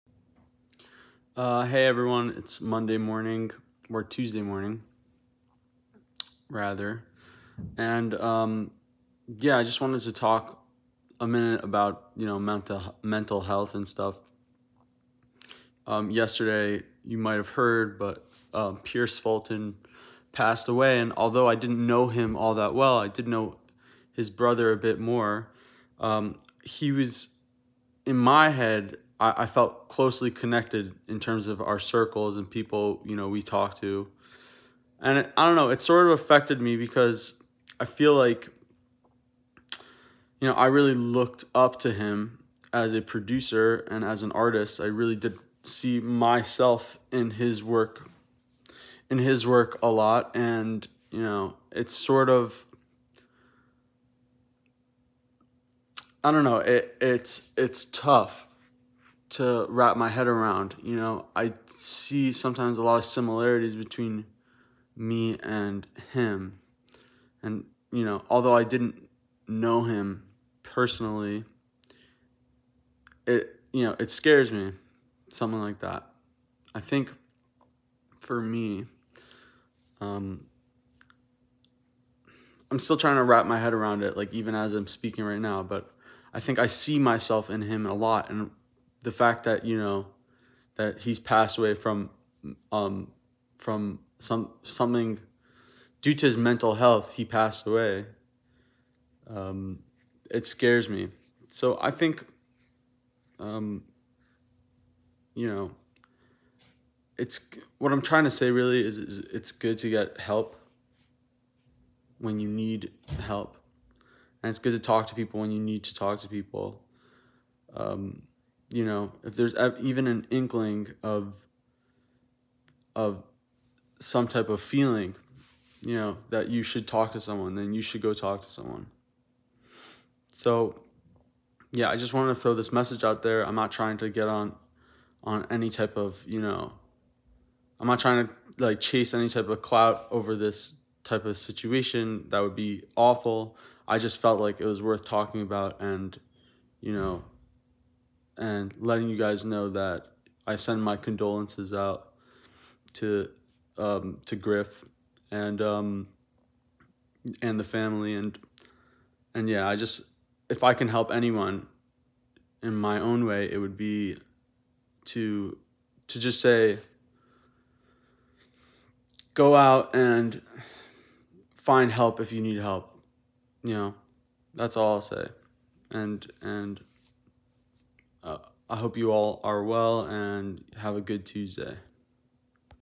About Mental Health...
Yesterday, Griff shared some awful news about the passing of his brother Pierce Fulton, and I wanted to speak about it briefly. Although I didn’t know Pierce myself, I aspired to work with him one day, and emulate his career. My condolences. RIP.